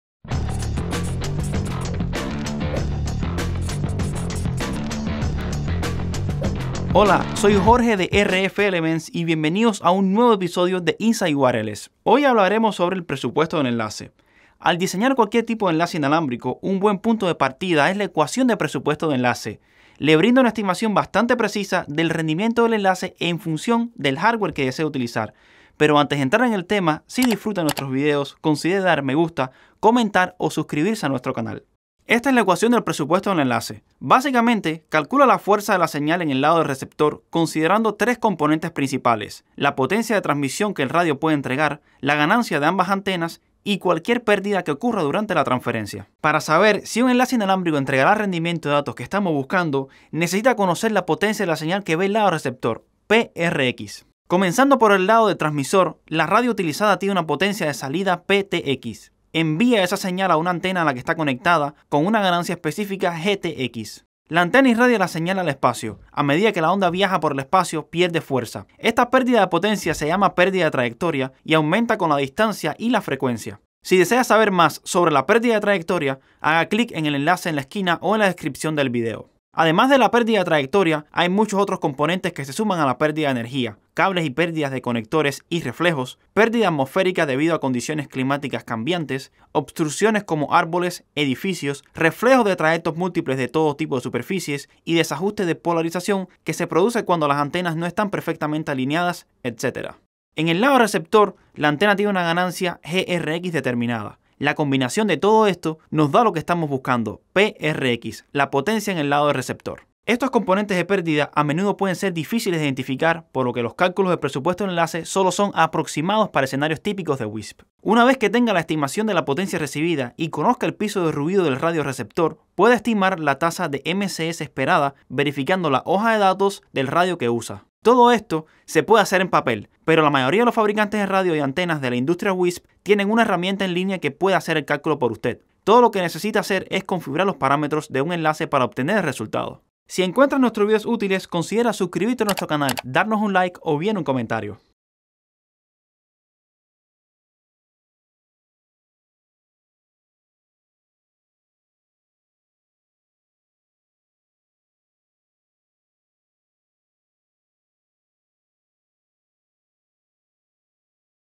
Inside Wireless en Español: Presupuesto de Enlace
Alfa y omega de cada planificación de enlace inalámbrico es la ecuación de presupuesto de enlace. ¿Cómo usarlo? ¿Cuáles son todos los componentes a considerar? En este video de Inside Wireless, aprenderá todo sobre el presupuesto de enlaces. Pruebe la calculadora de enlaces RF elements para verla en la práctica

La ecuación esencialmente calcula la potencia de una señal de RF en el lado del receptor considerando tres componentes principales:
- Energía proporcionada por el transceptor de radio
- Ganancias de antenas a ambos lados de un enlace
- Cualquier pérdida que se produzca durante la transferencia

El componente de pérdida se compone de muchas partes:
- Los cables + conectores introducen pérdida de potencia y reflexión
- Las condiciones atmosféricas disipan la energía de las olas dependiendo de la frecuencia de la señal
- Los reflejos de trayectos múltiples causan una atenuación aleatoria
- La falta de coincidencia de polarización también da como resultado una pérdida de energía

En realidad, a menudo se pueden ignorar partes de la pérdida mientras se mantiene una buena precisión del cálculo. Naturalmente, el mayor factor de pérdida es el más importante, por lo que si la pérdida de un cable es una fracción de dB, puede ignorarse.

Sin embargo, el cálculo del presupuesto del enlace siempre es una estimación que se puede hacer manualmente en papel, pero la mayoría de los fabricantes en la industria WISP tienen una calculadora de enlaces que hace los cálculos por usted con un nivel de detalle variable; algunos incluyen árboles, tal vez el clima. , pero muy pocos incluyen propagación por trayectos múltiples debido a la intensidad computacional al modelarlo.

0:00 Introducción
0:15 La ecuación
1:09 Componentes de pérdida
1:59 Conexión de tasa de pérdida y MCS
2:12 Calculadora de enlaces